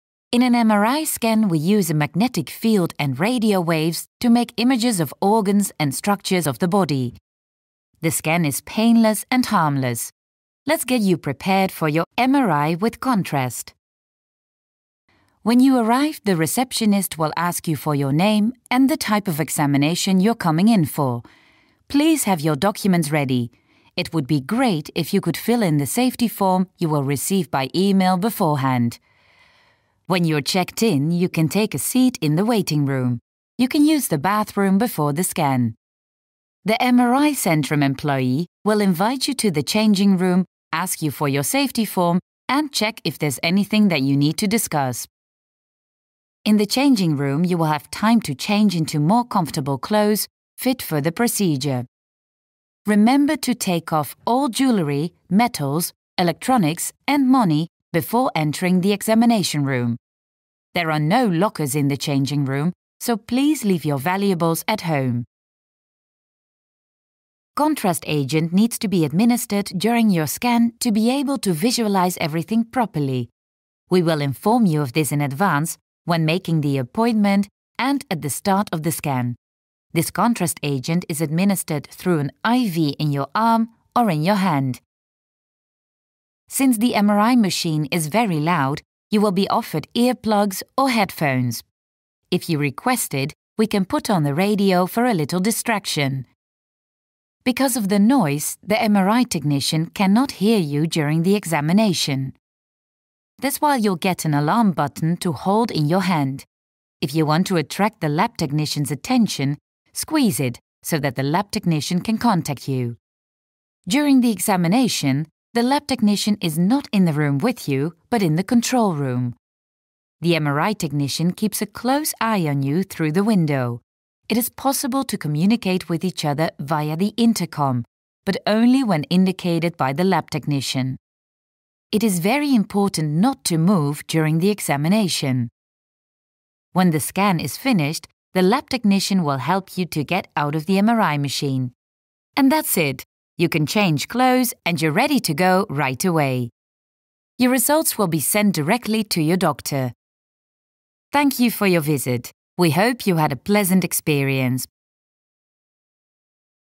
MRI Contrast 01
You are very welcome at the MRI Center for an MRI with contrast fluid. An MRI with contrast fluid is necessary for some examinations in order to obtain a good image of the area to be examined. With this video we prepare you for your MRI examination with contrast fluid.